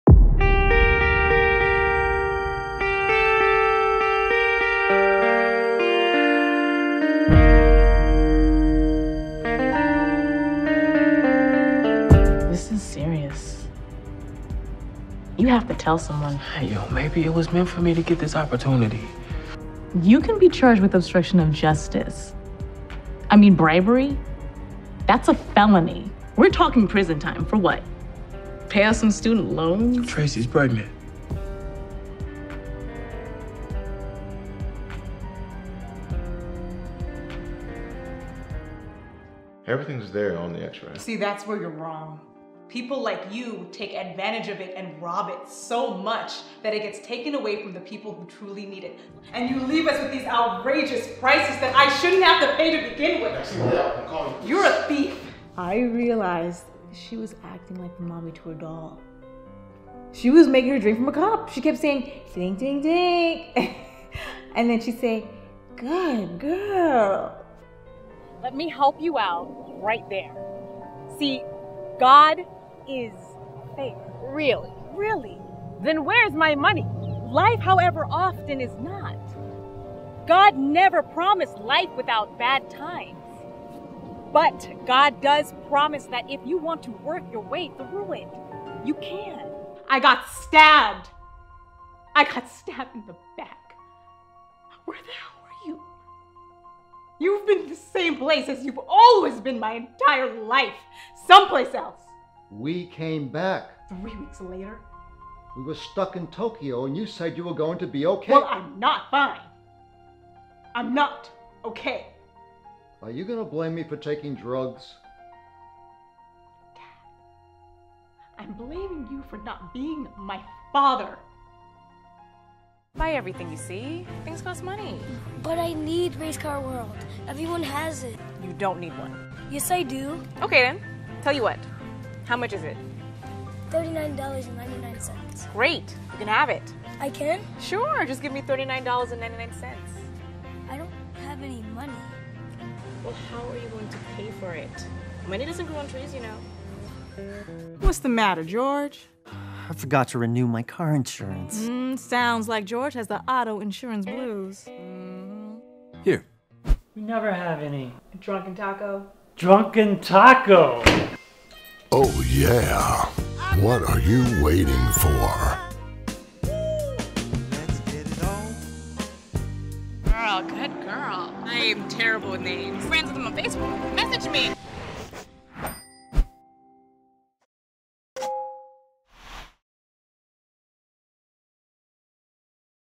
Lindsay Castillo - Acting Demo Reel
See why everyone is talking about actress and director Lindsay Castillo. With an in-demand look and uncommon talent, Lindsay has the experience and inspiration to light up the screen. See what Lindsay has been working on with highlights from some of her most engaging appearances and productions.